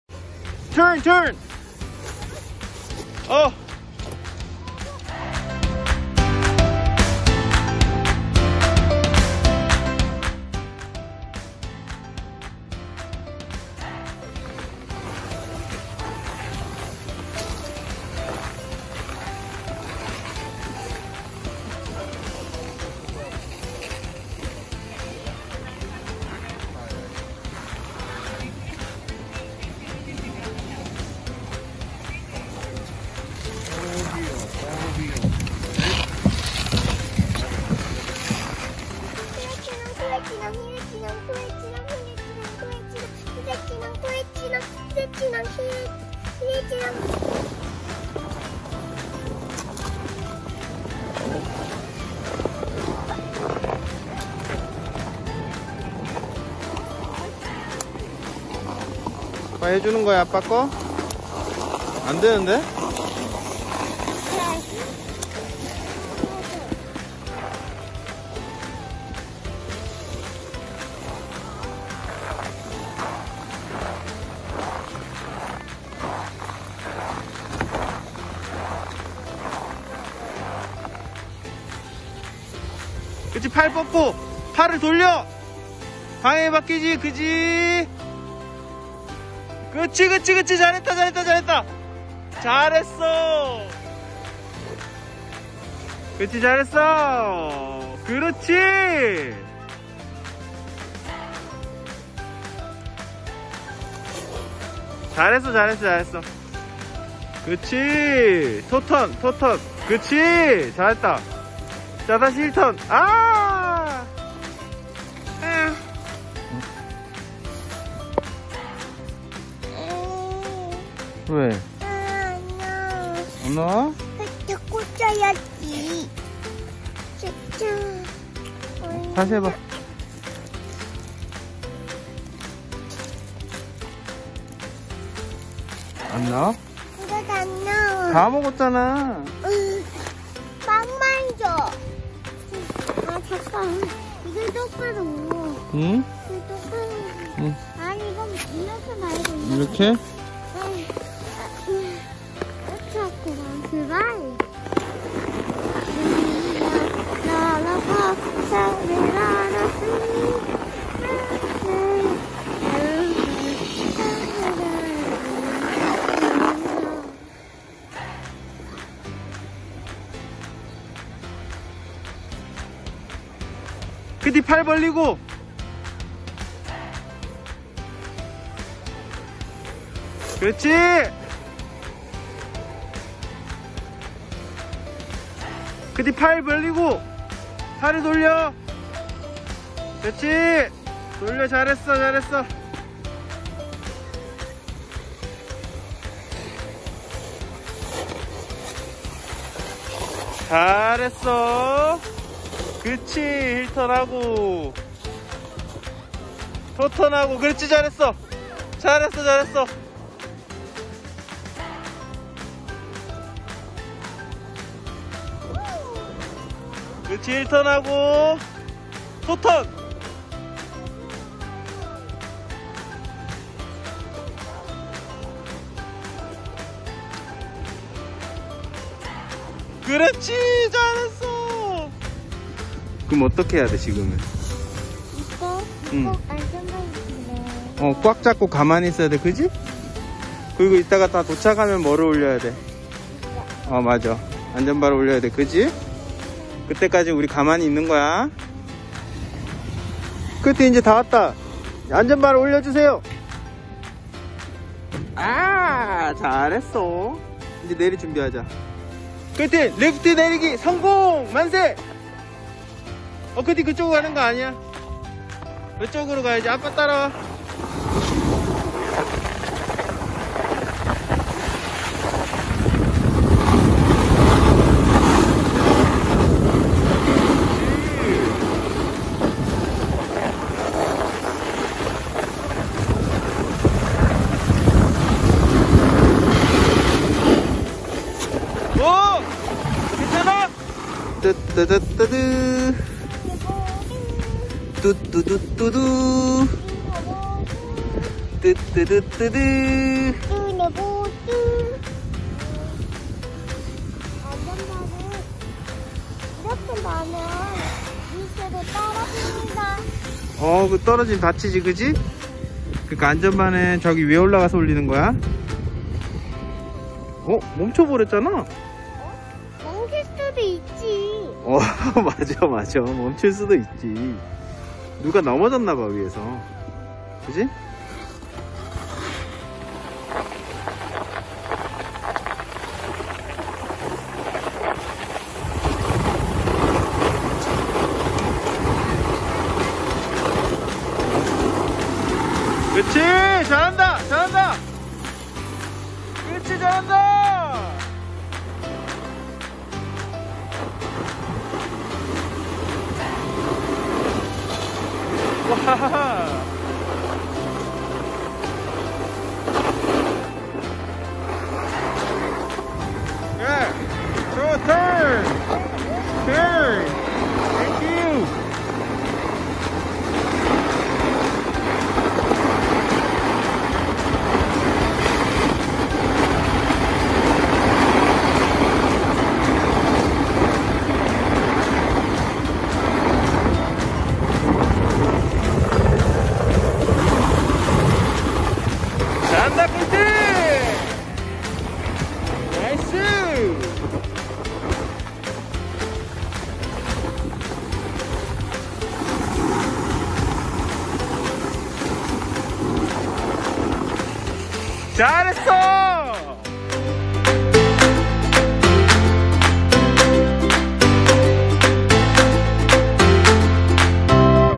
Kristen's Snowboarding (20/21) 7th Video
캐나다에 살고 있는 4살 클스틴의 스노보드 도전기 7번째 영상입니다.

레이크루이스에서 에드먼튼의 Rabbit Hill Ski Resort로 돌아왔어요!

지금까지 못했던 토턴을 오늘 과연 할 수 있을 것인가??!!